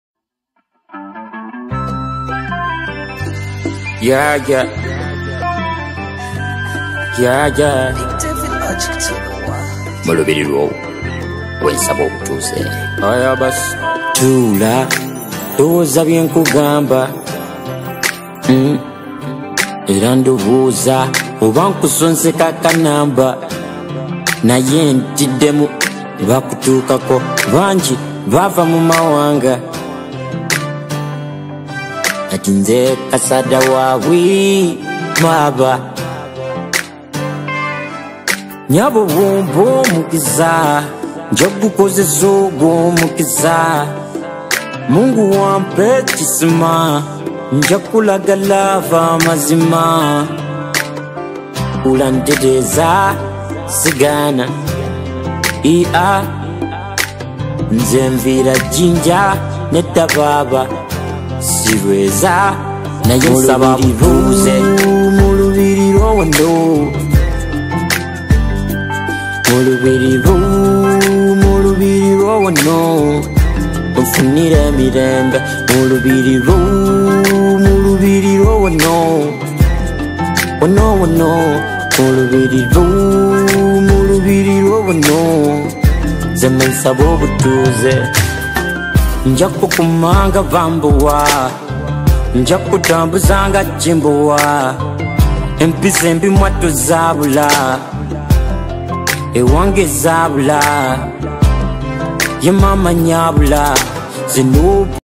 Drawing John blaq mulubili rwo
Pencil on paper drawing Ugandan musician john blaq